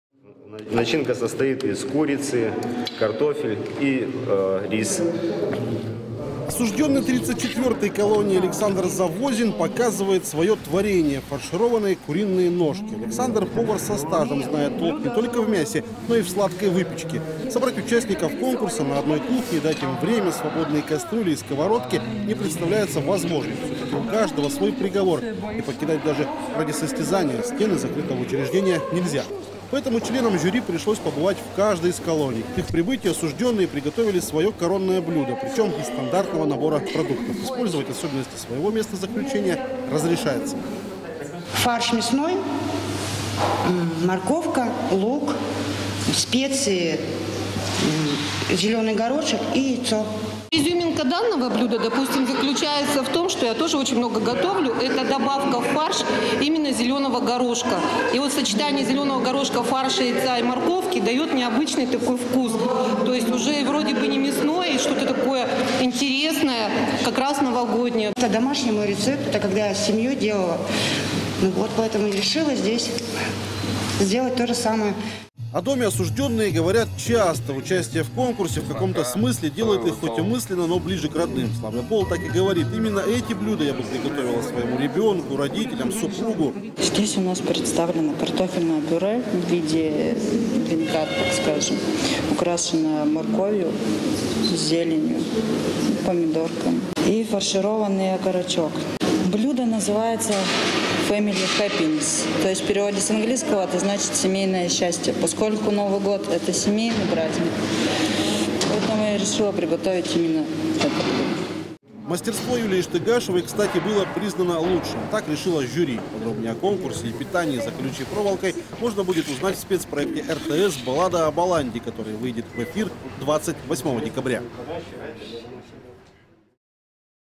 Заключенные в Хакасии состязались в кулинарном конкурсе
Куриный рулет, запеченный гусь, сельдь под шубой в виде роллов... В преддверии нового года участниками необычного кулинарного конкурса стали... осужденные, отбывающие наказание в исправительных учреждениях Хакасии. Соревнования подобного уровня в республике проходят нечасто.